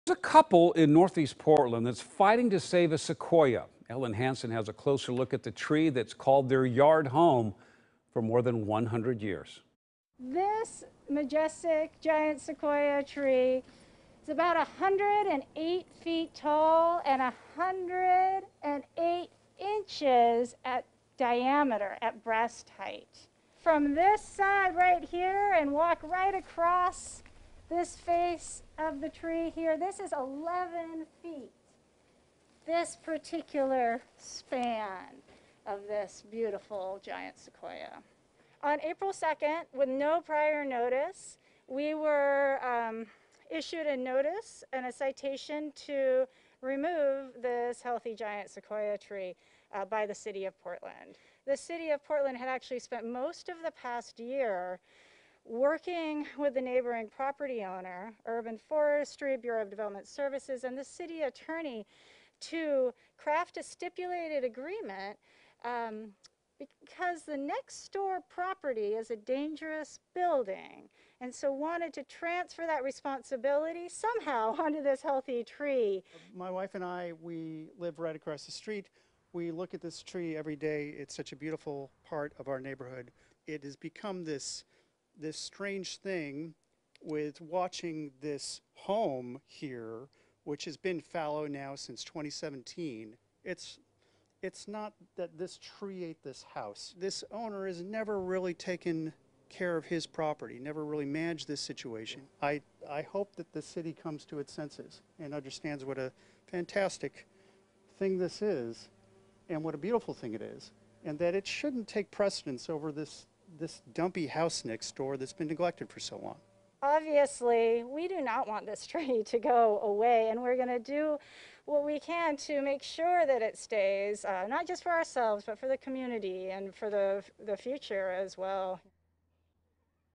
Portland couple fights to save 100-year-old giant sequoia tree
A Northeast Portland couple are fighting to save a giant sequoia tree in their yard.